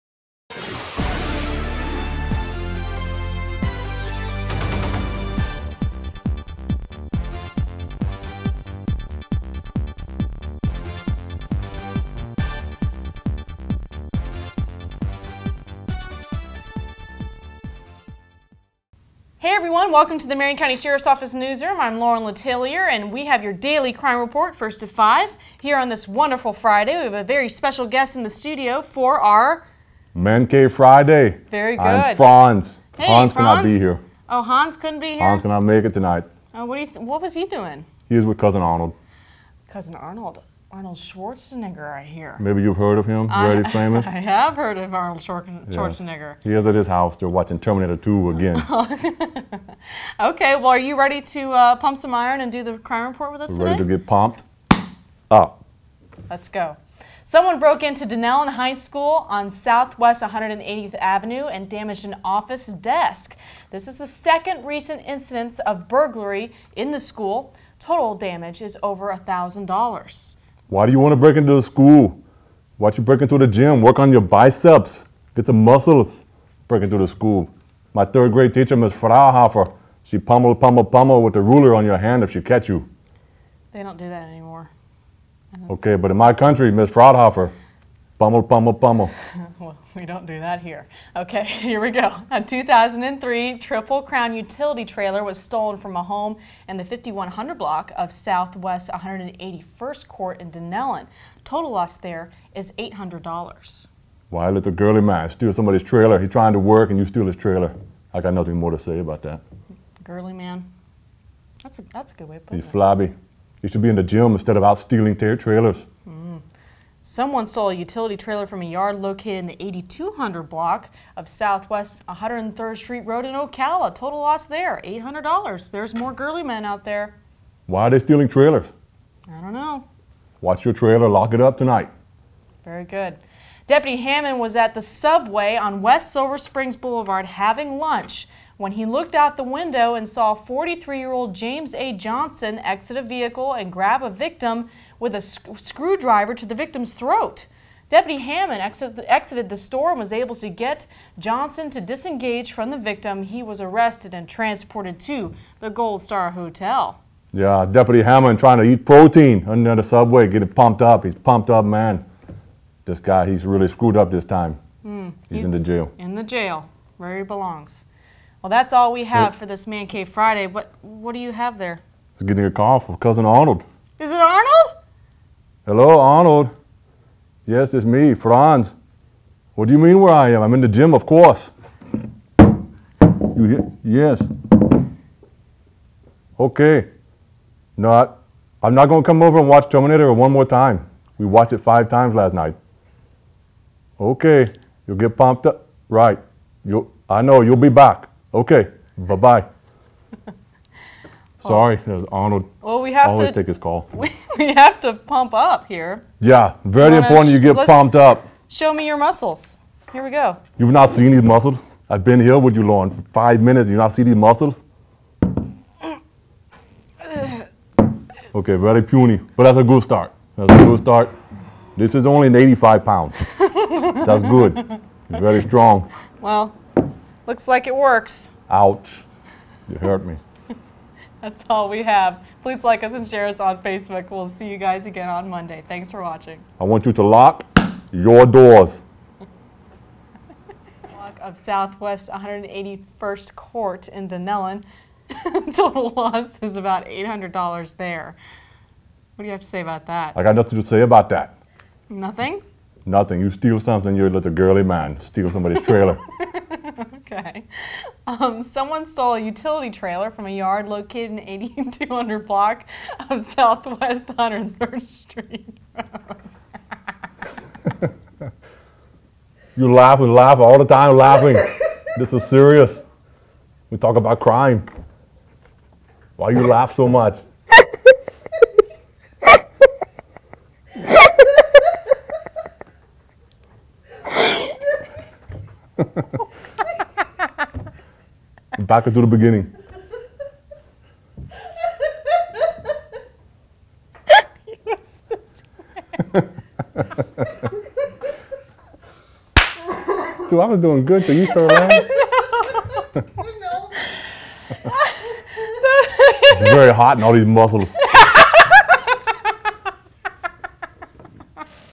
Your Daily Crime Report-First at Five (11-6-15)
Someone broke into Dunnellon High School on South West 180 Ave and damaged an office desk. This was the second recent incident of burglary to the school. Total damage is over $1,000

A 2003 Triple Crown utility trailer was stolen from a home in the 5100 block of South West 181st Court in Dunnellon.  Total loss is $800.

Someone stole a utility trailer from a yard located in the 8200 block of South West 103rd Street Road in Ocala.  Total loss there is $800.

Deputy Hammond was at the Subway on West Silver Springs Blvd having lunch when he looked out the window and saw 43 year old James A Johnson, exit a vehicle and grabbing the victim with a screwdriver to the victims throat.  Deputy Hammond exited the store and was able to get Johnson to disengage from the victim.  He was arrested and transported to the Gold Star Hotel.

That is all for this Friday.  Please like us and share us on Facebook.  We will see you guys again on Monday.